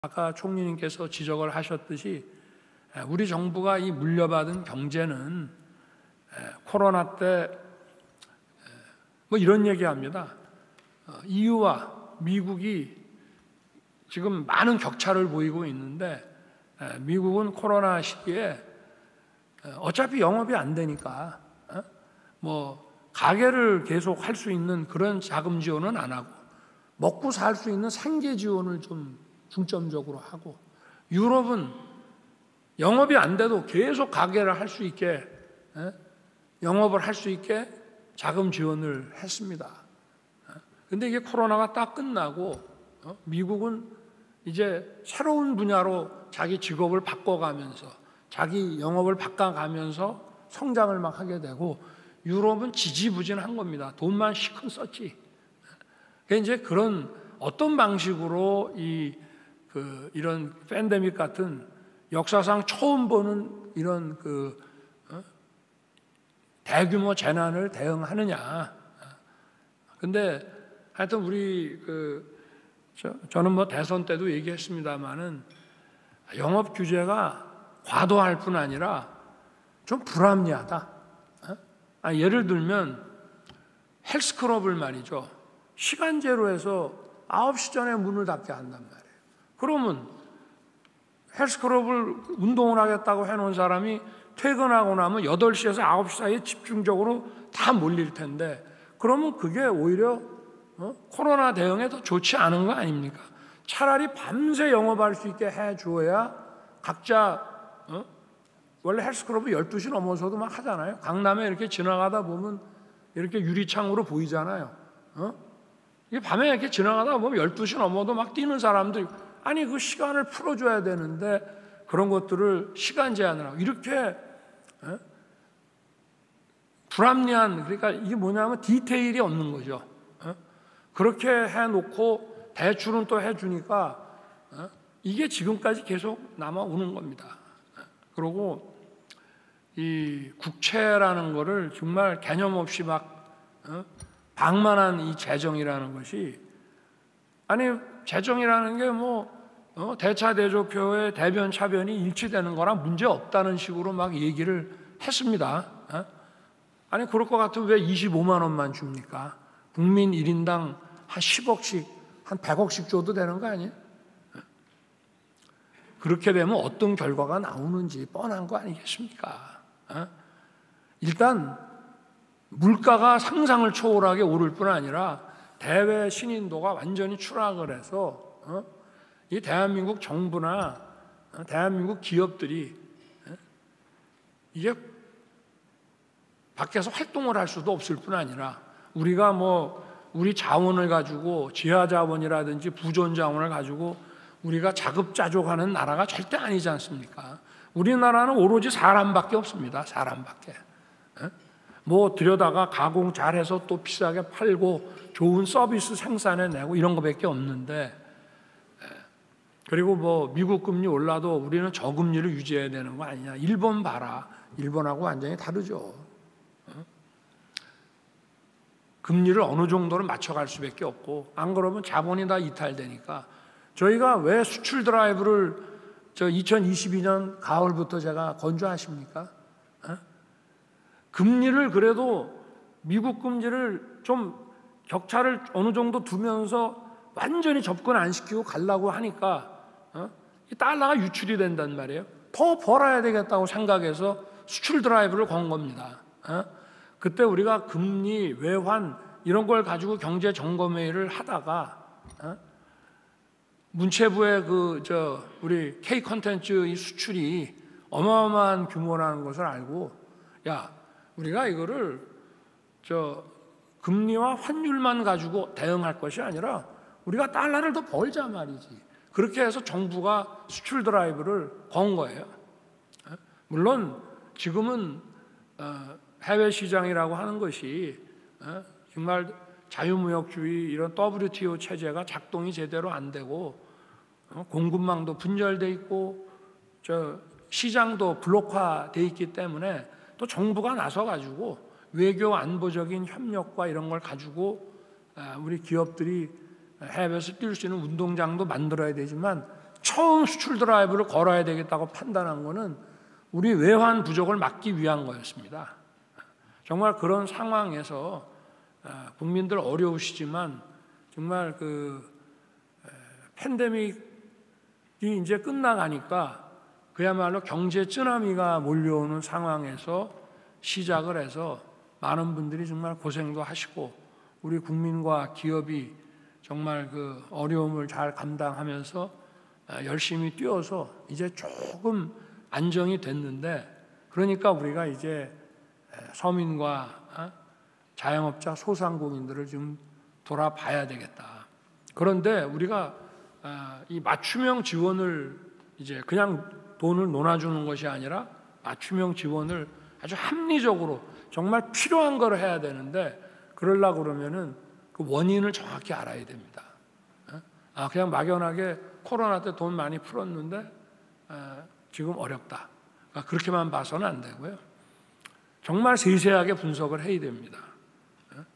[현장영상] "그럴 거면 왜 25만원만 줍니까?"…윤 대통령, 이재명 저격하더니 / JTBC News
윤 대통령이 오늘(3일) 오전 청와대 영빈관에서 열린 '하반기 경제정책 방향 및 역동경제 로드맵' 발표 회의에 참석했습니다.